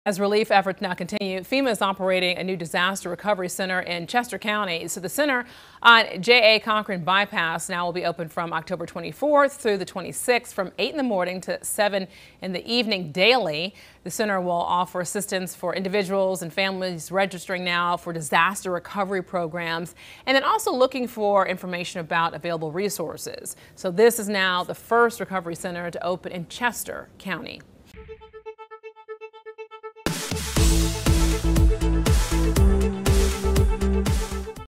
FEMA disaster center opens in Chester County
The recovery center will provide aid and assistance to those impacted by Hurricane Helene.